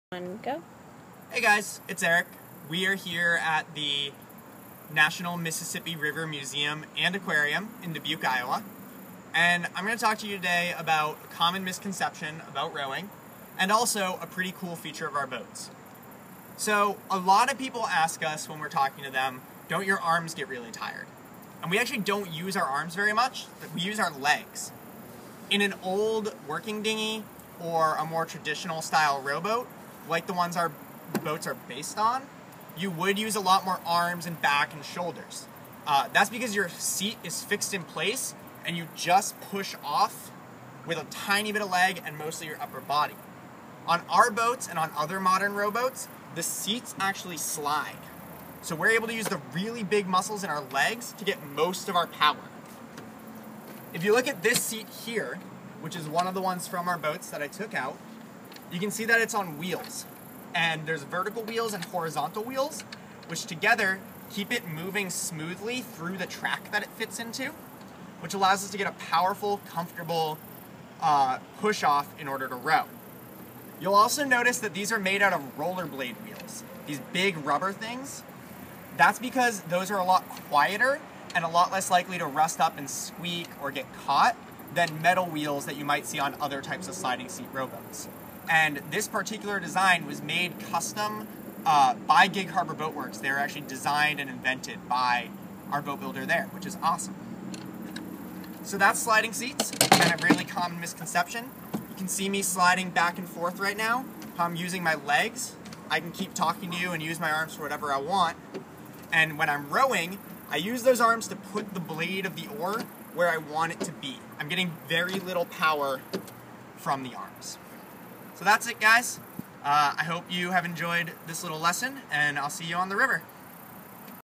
How Stuff Works: Sliding Seats
Join Eric to learn about one of the niftiest modern touches to our rowboats and why it's important to embrace the power of thighs.